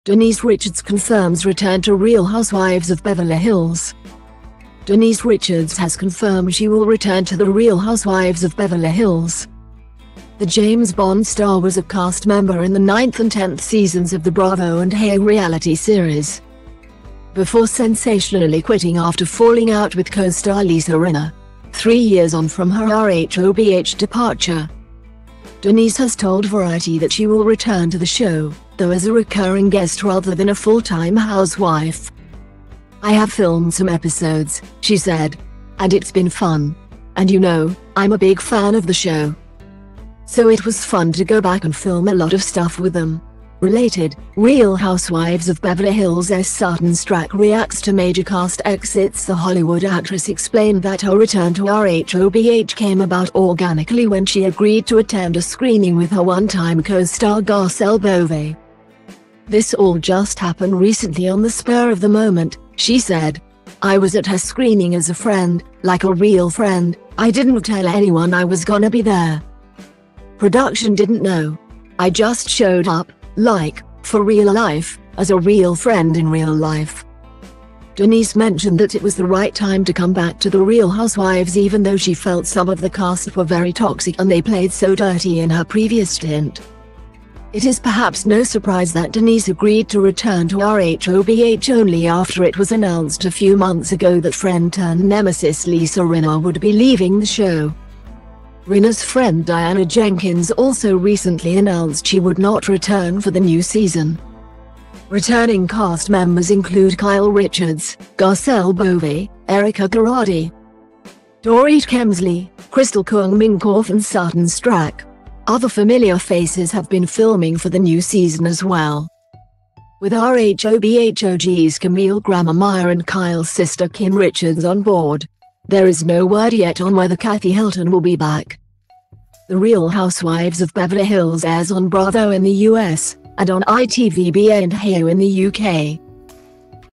Denise Richards confirms return to Real Housewives of Beverly Hills
Denise Richards has confirmed she will return to The Real Housewives of Beverly Hills .The James Bond star was a cast member in the ninth and tenth seasons of the Bravo and hayu reality series,before sensationally quitting after falling out with co-star Lisa Rinna . Three years on from her RHOBH departure,